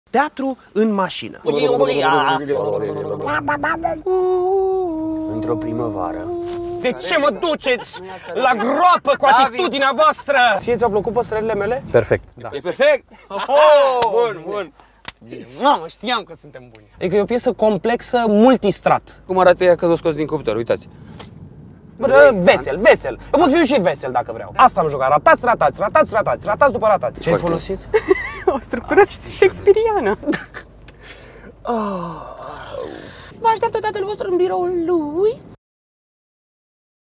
Teatrul ING. Trailer
Piesa “Doi frati” a avut prima reprezentatie in masina pentru cei patru clienti ING – castigatori in cadrul proiectului Teatrul ING. Daca n-ai avut norocul sa iei un loc pe bancheta din spate, poti sa vezi piesa pe micile ecrane. Intra pe ca sa vezi piesa in 3 acte.